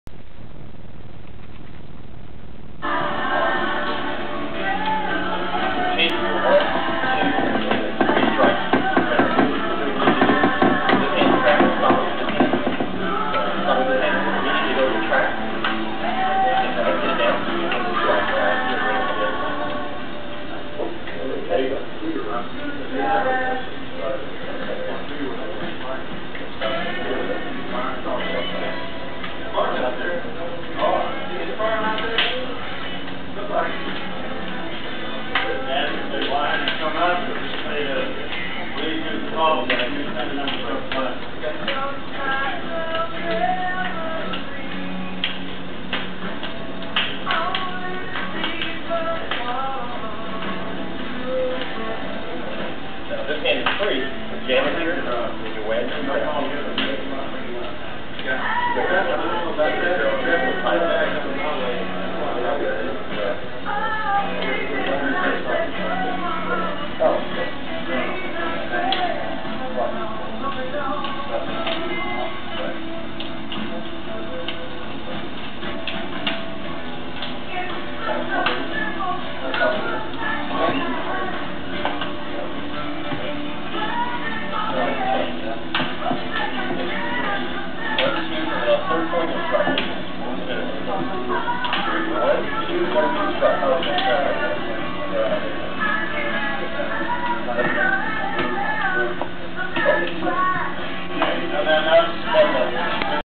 Oniyama training at the Kamiyama Dojo #1 09-18-99
Eddie Moore sensei working with Kevin Gutierrez on a trapping drill on a chill Saturday morning Art of Combat (AOC) class on September 18, 1999.  Kevin's first AOC class at the Kamiyama Dojo ever to attend.